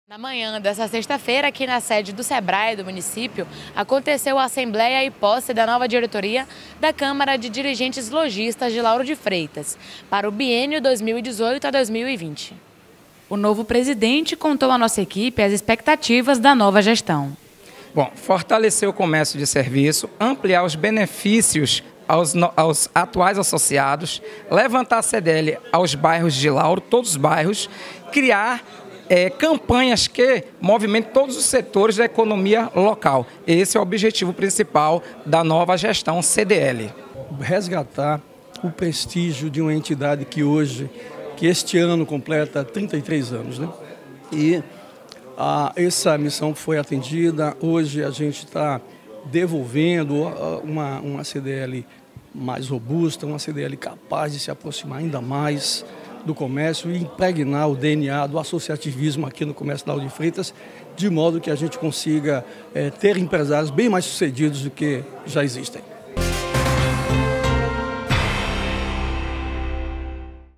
CDL Lauro de Freitas
Na manhã desta sexta-feira, em Assembleia, foi apresentada a nova diretoria da CDL Lauro de Freitas.